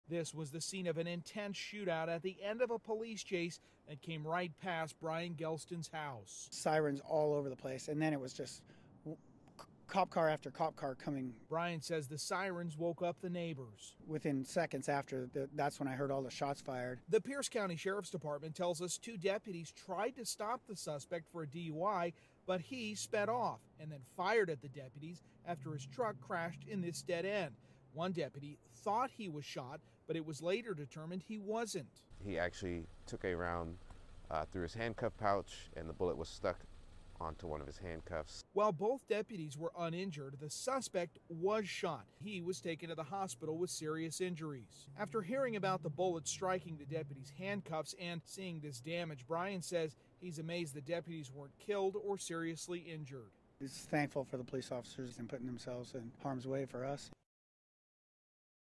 Handcuffs save deputy from gunfire after deflecting bullet
A man suspected of DUI was taken to the hospital with serious injuries after a shootout with Pierce County Sheriff's Department deputies in Tacoma early Wednesday morning.

About four minutes after spotting and attempting to pull over the suspect's vehicle, Sgt. Darren Moss Jr. said he and another deputy called out that the man started shooting from the car and they had returned fire.

One of the deputies initially thought he had been shot and was checked out by medics, but was uninjured. Moss said the deputy’s partner later discovered at the precinct he was hit by the gunfire, but the bullet struck the handcuffs on his belt and was stopped.

“He was hit in the pouch. The bullet hit one of his handcuffs and was lodged in the pouch,” Moss told KOMO News crews at the scene Wednesday morning. “It didn’t make it through into the vest, but obviously a very close call. Probably the reason he couldn’t feel it is that it hit the metal on his handcuff case and didn’t actually strike the vest or his body.”

00:00 Bullet hits Pierce County deputy's handcuff pouch during standoff, shooting with DUI suspect
02:30 2 children, 1 adult taken to hospital after Lakewood apartment fire
04:04 end